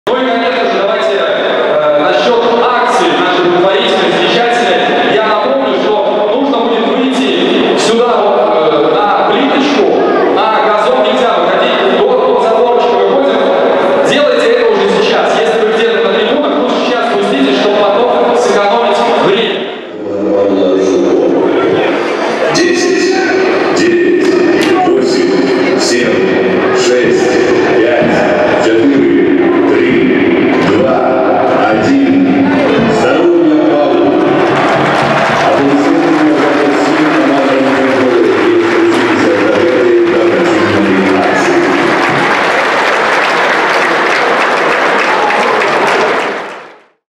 Акция "Шарик надежды" на "Центральном"
Перед игрой с БАТЭ футбольный клуб «Гомель» совместно с волонтёрским отрядом «Открытые сердца» провели благотворительную акцию.

Возле стадиона любой желающий мог приобрести воздушный шарик, который за несколько минут до начала встречи по команде диктора нужно было отпустить в воздух.

Все вырученные деньги от продажи шаров, а также часть от проданных билетов на матч пойдут на помощь пятилетнему Павлу Шурпо из Речицы, которому требуется сверхсрочный сбор на обследование и лечение.